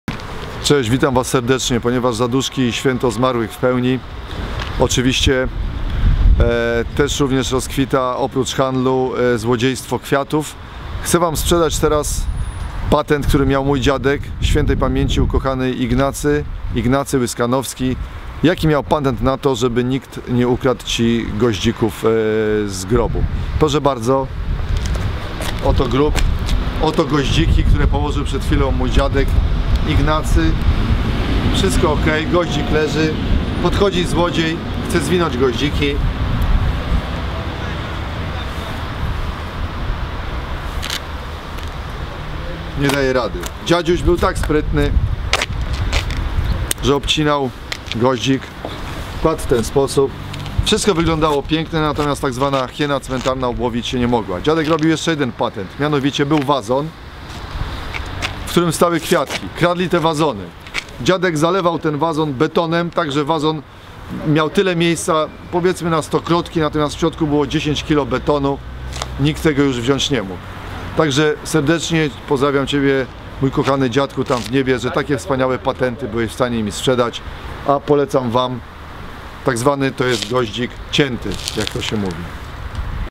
Sposób na cmentarnych złodziei
Mój ukochany dziadek miał na imię Ignacy. W Zaduszki i Wszystkich Świętych, wspominam Go pokazując Wam, jaki miał patent na cmentarnych złodziei kwiatów.
Szymon